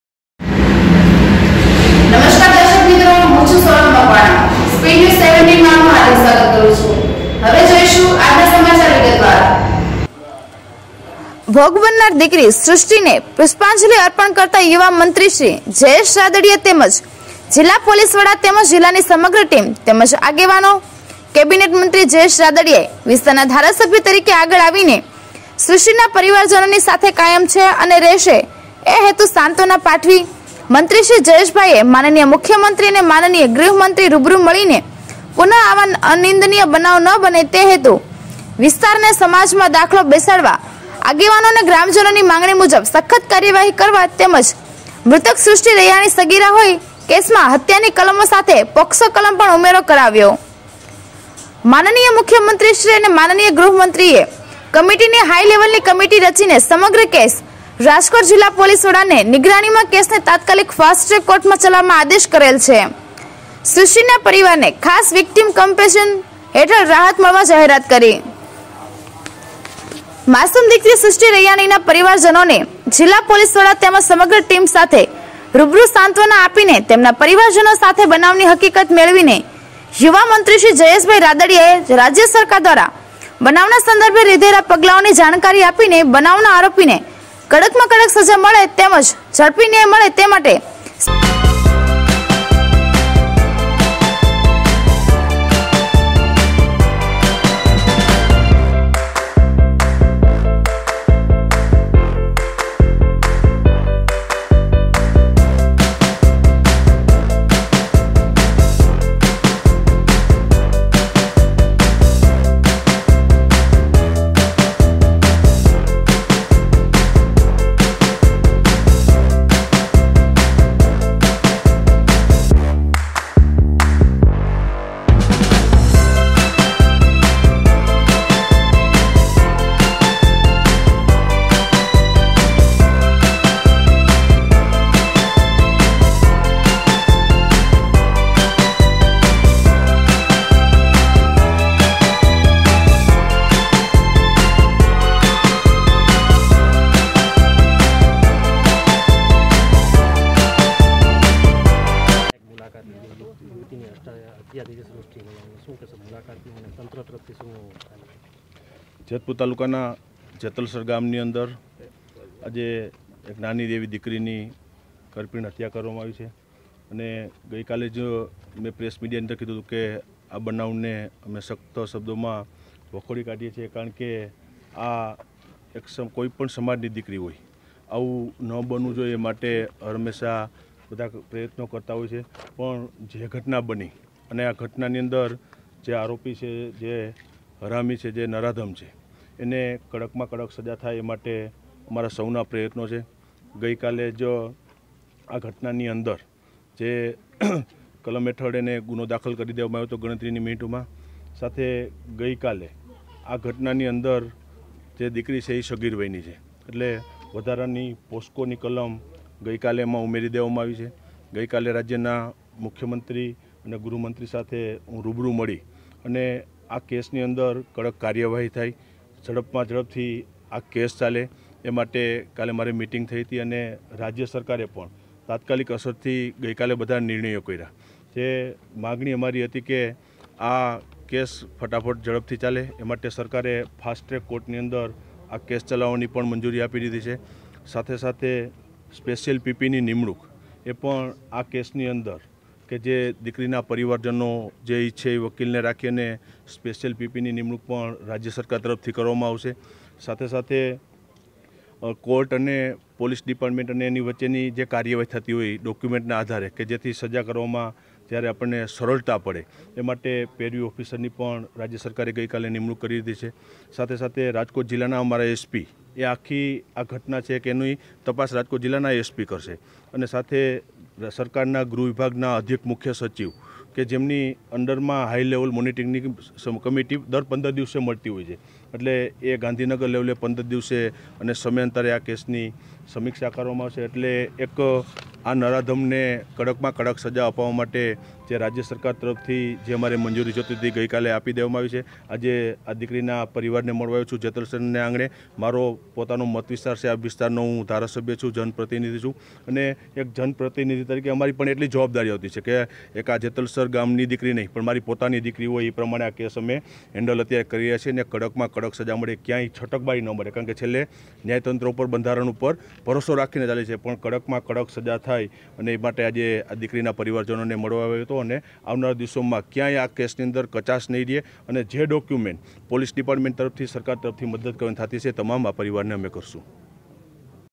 RAJKAML જેતલસર સગીરાની હત્યા કેસ મામલે આરોપી પર મજબૂત સકંજો કસાયો...
જેતલસર સગીરાની હત્યા કેસ મામલે આરોપી પર મજબૂત સકંજો કસાયો...

કેબિનેટ મંત્રીએ આપી બાંહેધરી જલ્દીથી કાર્યવાહી હાથ ધરાશે.

ભોગ બનનાર દીકરી “સૃષ્ટી”ને પુષ્પાજંલી અર્પણ કરતા યુવા મંત્રીશ્રી જયેશ રાદડીયા તેમજ જીલ્લા પોલીસ વડા તેમજ જીલ્લાની સમગ્ર ટીમ તેમજ આગેવાનો.

કેબિનેટ મંત્રી જયેશ રાદડિયાએ વિસ્તારના ધારાસભ્ય તરીકે આગળ આવી શ્રુષ્ટિના પરિવારજનોની સાથે કાયમ છે  અને રહેશે એ હેતુ સાંત્વના પાઠવી, મંત્રીશ્રી જયેશભાઇએ માન.મુખ્યમંત્રી અને માન.ગૃહમંત્રીને રૂબરૂ મળીને પુન: આવા નિંદનીય બનાવ ન બને એ હેતુ વિસ્તાર અને સમાજમાં દાખલો બેસાડવા આગેવાનો અને ગ્રામજનોની માંગણી મુજબ સખત કાર્યવાહી કરવા તેમજ મૃતક શ્રુષ્ટિ રૈયાણી સગીરા હોય કેસમાં હત્યાની કલમો સાથે પોકસો કલમ પણ ઉમેરો કરાવ્યો.

માન.મુખ્યમંત્રીશ્રી અને માન.ગૃહમંત્રીએ કમિટીની હાઇલેવલની કમિટી રચીને સમગ્ર કેસ રાજકોટ જિલ્લા પોલિસ વડાને નિગરાણીમાં કેસને તાત્કાલિક ફાસ્ટ્રેક કોર્ટમાં ચલાવવા આદેશ કરેલ છે. શ્રુષ્ટિના પરિવારને ખાસ વિકટીમ કંપેનસેશન હેઠળ રાહત મળવા જાહેરાત કરી.

માસુમ દીકરી “સુષ્ટી રૈયાણી”ના પરિવારજનોને જીલ્લા પોલીસ વડા તેમજ સમગ્ર ટીમ સાથે રૂબરૂ સાંત્વના આપીને તેમના પરીવારજનો સાથે બનાવની હકીકત મેળવીને યુવા મંત્રીશ્રી જયેશ રાદડીયાએ રાજ્ય સરકાર દ્વારા બનાવના સંદર્ભે લીધેલા પગલાઓની જાણકારી આપીને બનાવના આરોપીને કડક મા કડક સજા મળે તેમજ ઝડપી ન્યાય મળે તે માટે સ્પે.સરકારી વકીલની નિમણુંક કરાવીને સગીર દીકરી હોય જેથી“પોસ્કો”કલમ લગાડીને આ કેસ ફાસ્ટટ્રેક ચલાવવાની સાથે સમગ્ર કેસનુ સુપરવીઝન જીલ્લાના એસ.પી.શ્રી દ્વારા કરવામા આવશે તેમજ રાજ્ય સરકારના ગૃહ વિભાગના અધિક મુખ્ય સચિવશ્રી દ્વારા દર ૧૫ દિવસે મળતી હાઈપાવર કમીટીમા આ કેસનો રિવ્યુ લેવામા આવશે જેથી માસુમ દીકરીને ઝડપી ન્યાય મળી રહે તેવા અમારા સક્રિય પ્રયાસો રહેશે તેવી ખાત્રી પણ મંત્રીશ્રી જયેશ રાદડીયાએ સમગ્ર ગ્રામજનો તેમજ દીકરીના પરિવારજનોને આપી હતી.